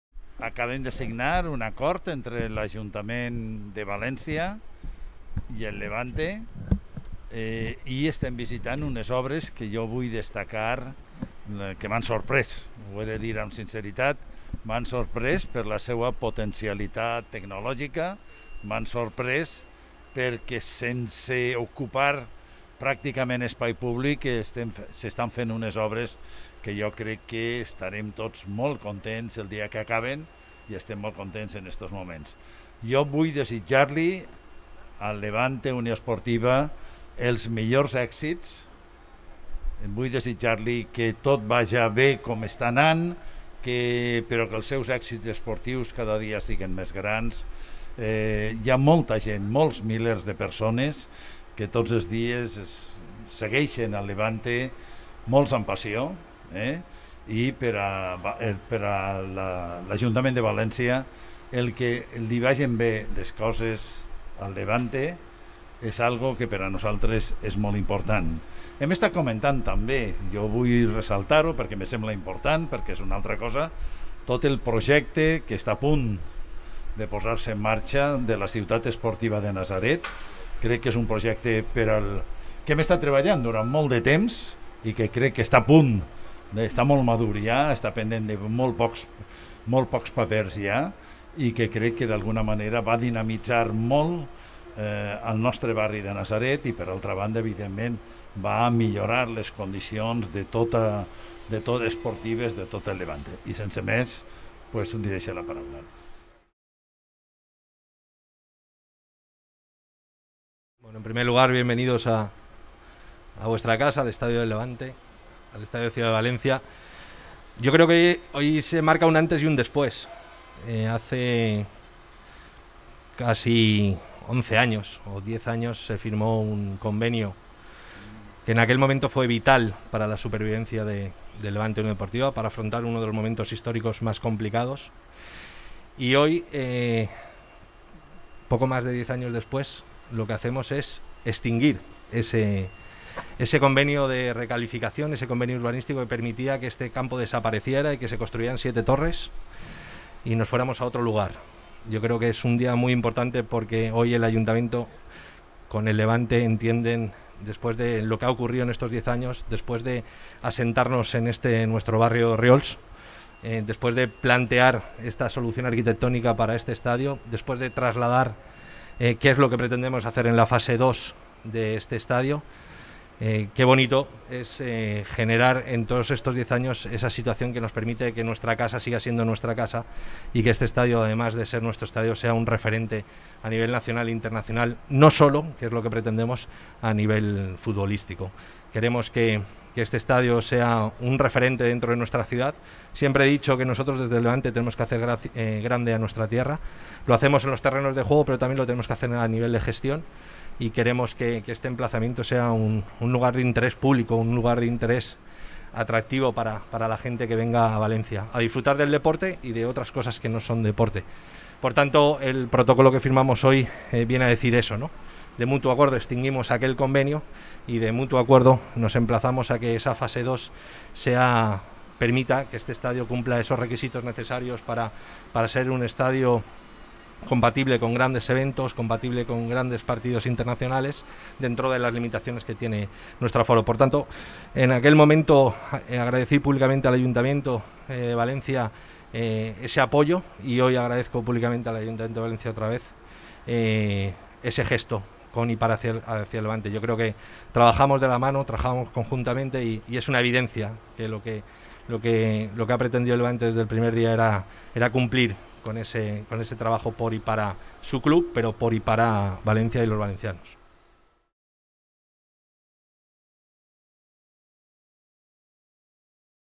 L'alcalde, Joan Ribó, visita les obres de l'Estadi Ciutat de València
L'alcalde i el president del @LevanteUD signen el Protocol d'Intencions
entre el consistori i l'entitat esportiva per a la reforma de l'Estadi Ciutat de València, que garanteix el manteniment en el lloc actual, la reforma de l'estadi, i els terminis d'execució de les obres.